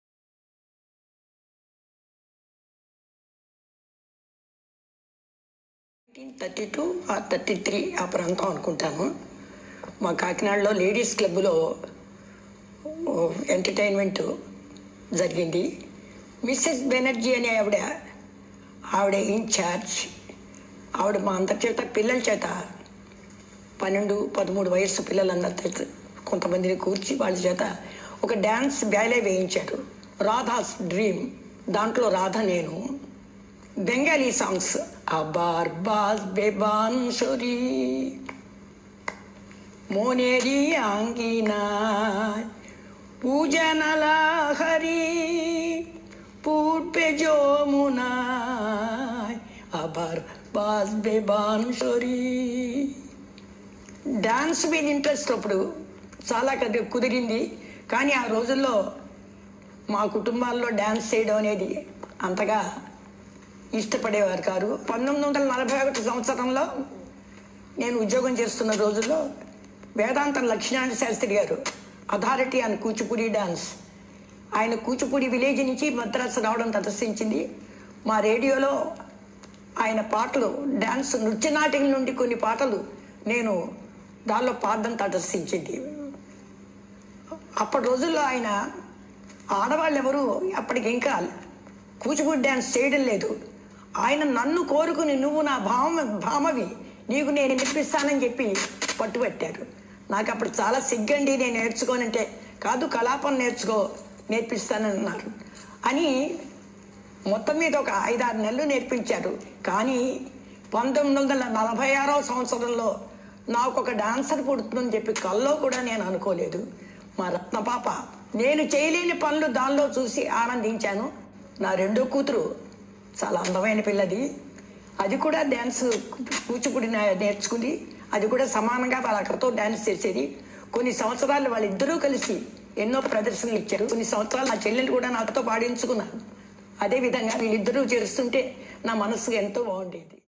Vinjamuri Anasuya Devi snippets | Dance
A small snippet about Anasuya’s love for dance